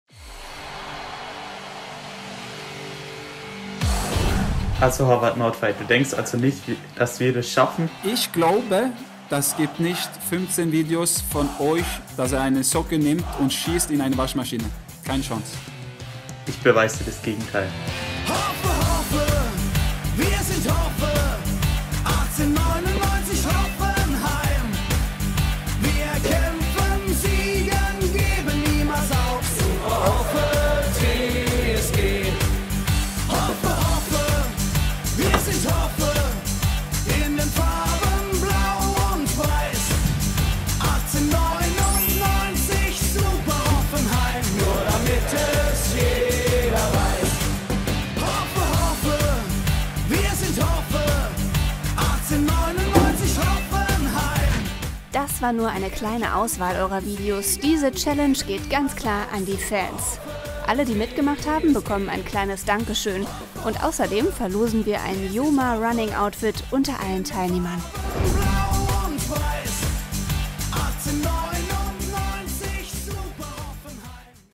Türchen 15 | Auflösung
Joma präsentiert Türchen 15 der TSG-Adventschallenge. Darin habt Ihr unseren Norweger Håvard Nordtveit geschlagen. Glückwunsch! Alle Teilnehmer*innen erhalten ein Dankeschön. Außerdem verlosen wir unter allen Teilnehmer*innen ein Running Outfit von JOMA. Wir melden uns bei Euch! Der neue Spielstand lautet: 13:3 für die Fans.